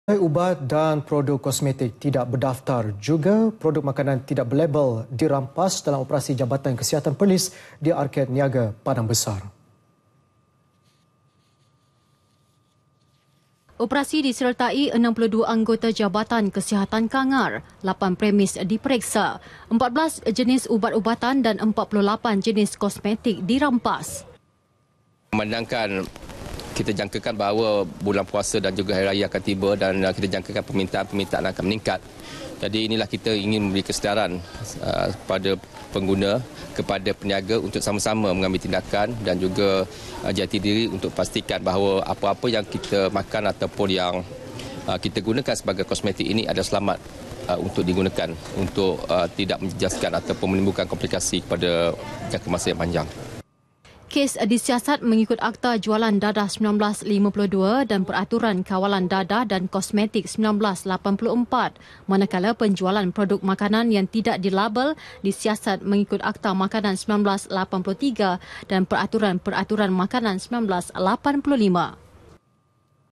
RAMPAS UBAT, KOSMETIK TIDAK BERDAFTAR - ARKED NIAGA PADANG BESAR DIPERIKSA [12 MEI 2016]
Pelbagai ubat dan produk komestik tidak berdaftar...juga produk makanan tidak berlabel, dirampas dalam operasi Jabatan Kesihatan Perlis di Arked Niaga Padang Besar.